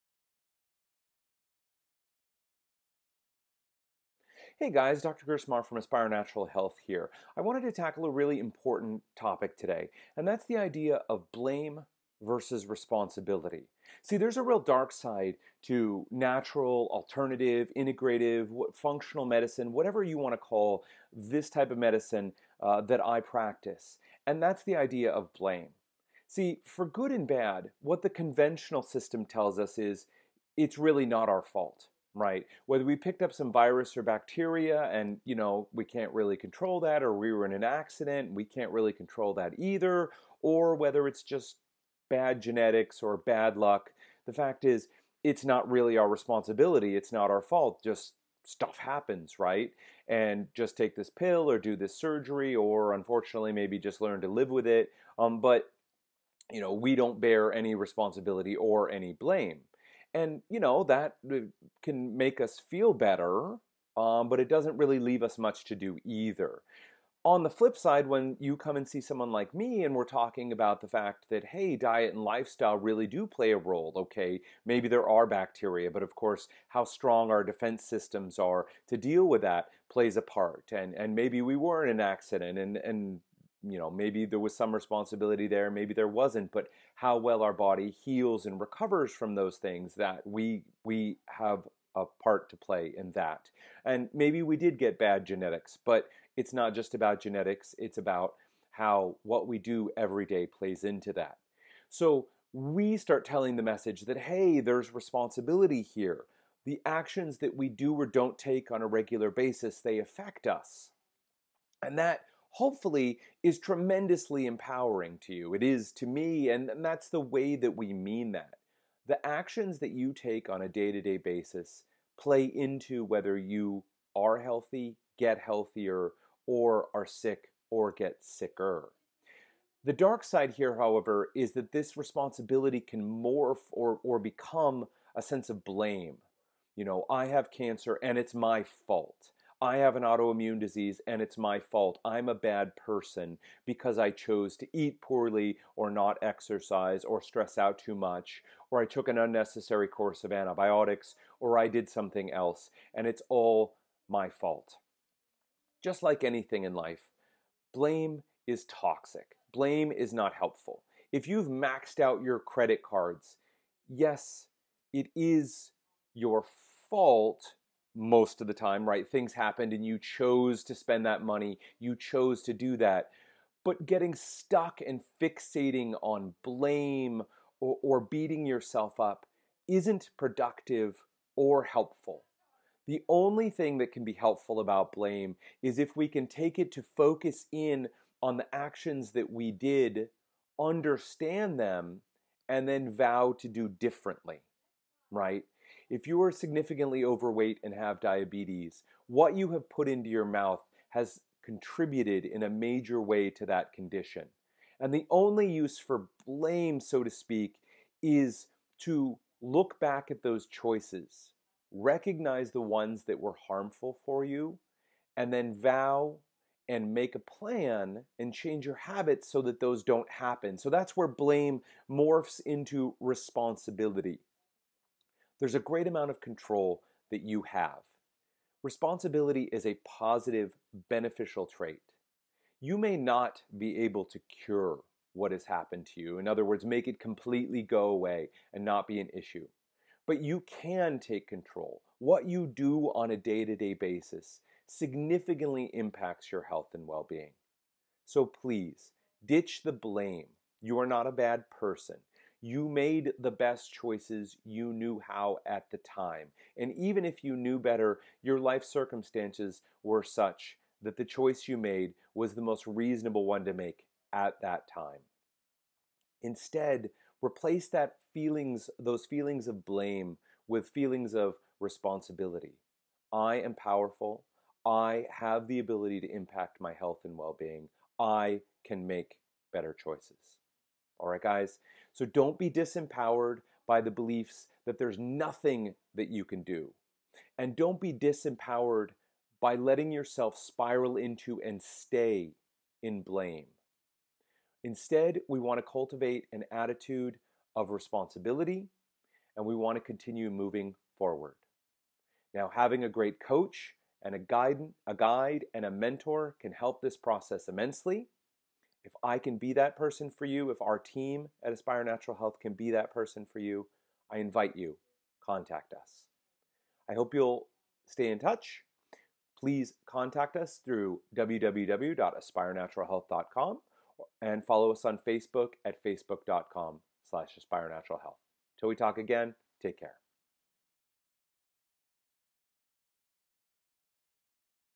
ANH Vlog Blame vs Responsibility
If you're receiving natural or integrative care, or thinking about it, you need to watch this video.

Perhaps the biggest dark side to the whole natural / integrative / holistic / functional medicine field is blame.  In this video Dr. Gerstmar talks about blame and why it hurts you.  He also discusses the critical importance of moving through blame to responsibility.